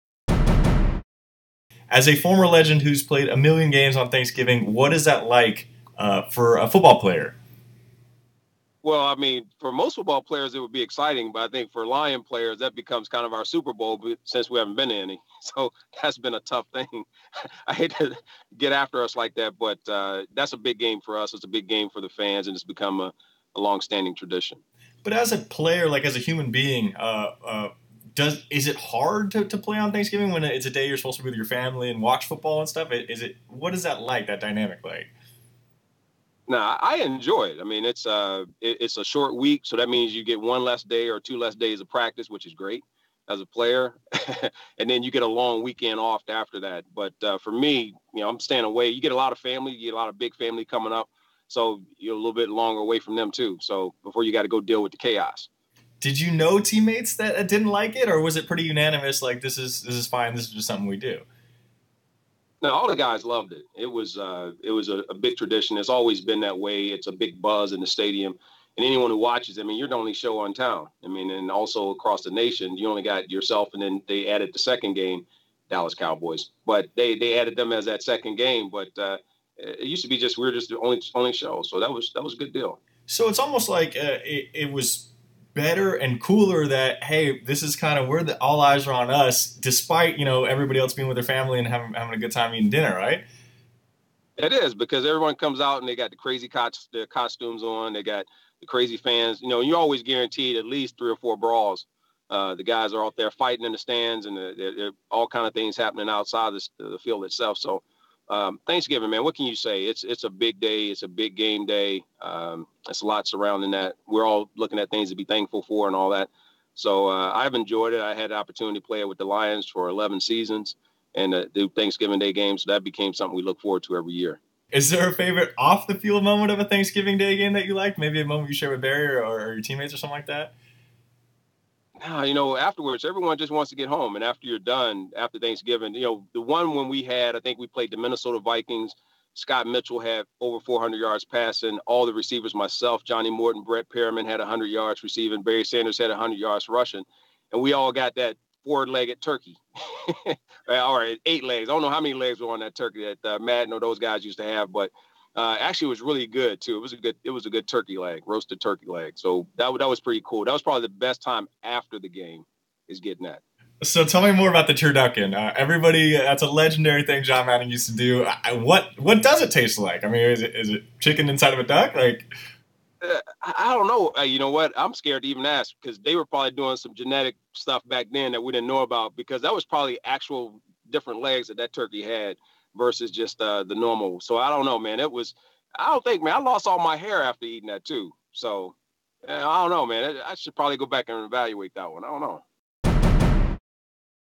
Herman Moore Says Lions Love Playing On Thanksgiving
Here's an age-old NFL question ... does it suck to be away from family playing on Thanksgiving -- or is it flat-out cool being a part of Turkey Day tradition??